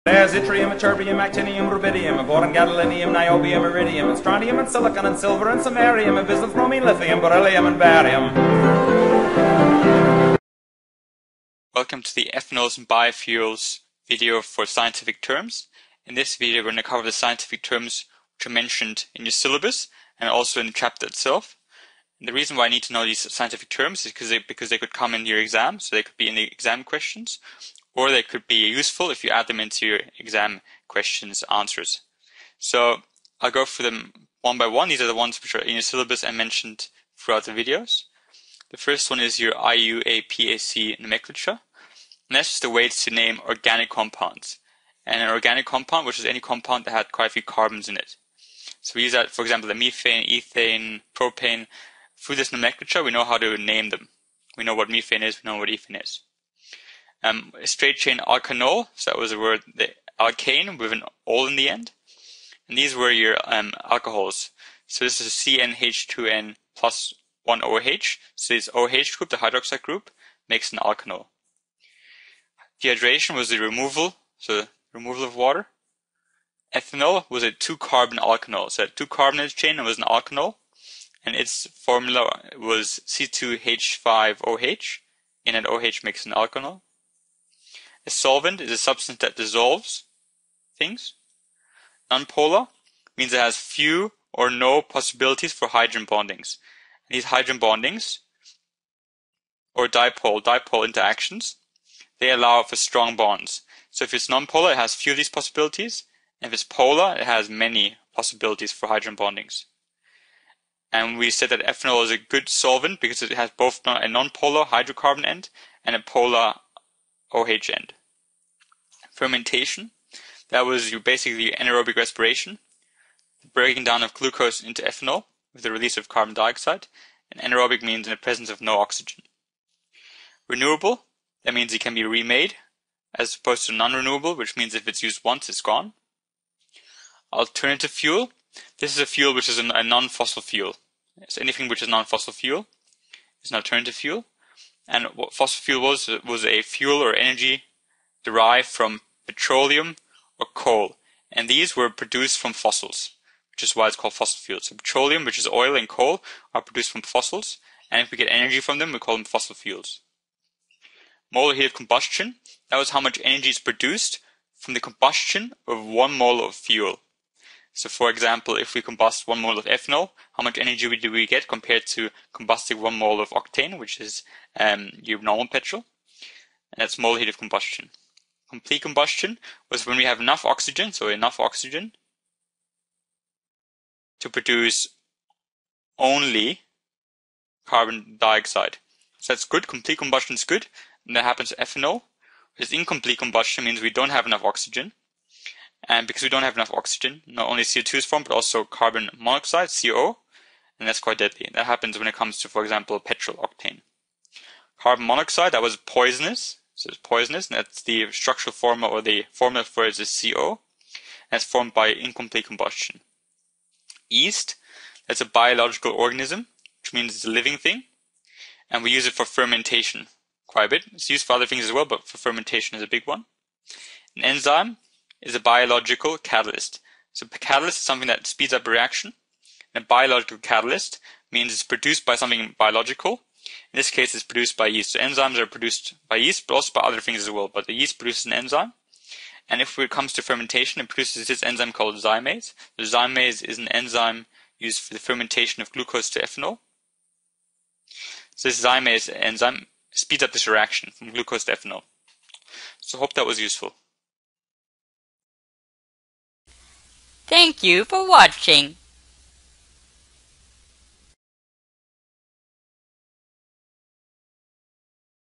Scientific terms (ethanol and biofuels chapter)
Covers all important scientific terms of the ethanol and biofuels of the production of material module